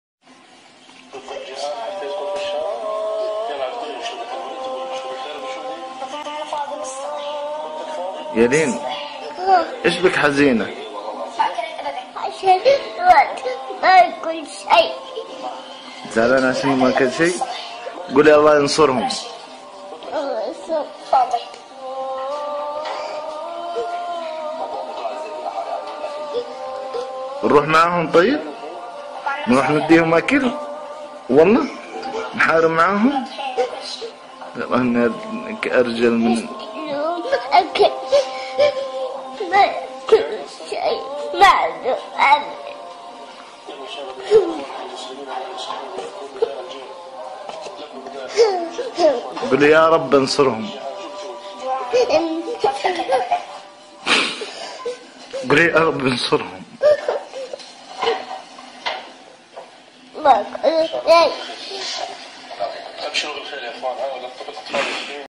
طفله تبكي بعد مشاهدتها معاناة الاطفال في سوريا
فيديو لطفلة يبدو أنها من بلد خليجي وهي تذرف الدموع تعاطفا مع أطفال سورية، وذلك بعد أن شاهدت في هاتف ذكي "ولد ما ياكل شي"، وقد ظهر الحزن الشديد على وجهها البريء. لم تكف الطفلة عن البكاء بحرقة وهي تدعو الله بأن ينصر الأطفال السوريين، حتى أن الصغيرة التي تعاطفت مع أقرانها بالفطرة، بعيدا عن معطيات سياسية لا تفهمها، أبدت رغبة بالتوجه إلى سورية للقتال من أجلهم، وأن تجلب لهم الطعام، وقد رفعت رأسها تضرعا إلى الله.